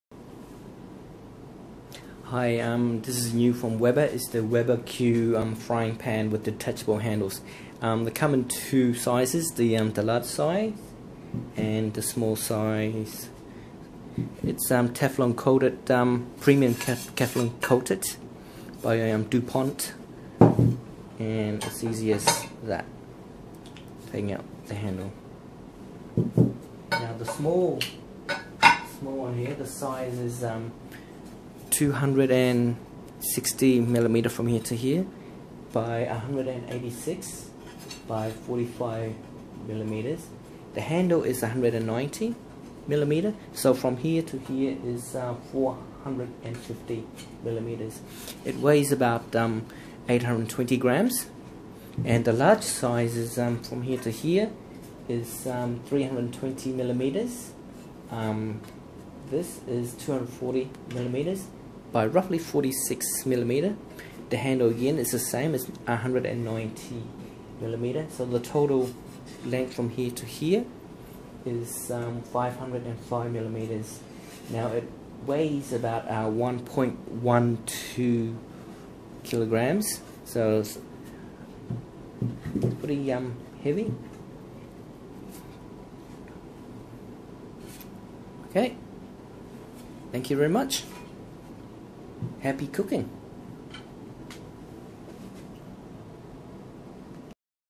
Weber Q Ware Frying Pan with Detachable Handle
Weber Q Ware Frying Pan
Now you really can cook everything outside on your Weber Q. The Q Ware frying pans allow you to cook omelettes, schnitzels, potato bakes, desserts and more! The Platinum Teflon coated surface is easy to clean and the detachable handle makes it easy to take the pans on and off your Weber Q.

Small (not including handle): 260mm (450mm with handle) x 186cm x 45mm
Handle length: 190mm
weight: 820g

Large (not including handle): 320mm (505mm with handle) x 240cm x 46mm
Handle length: 190mm
weight:1.120kg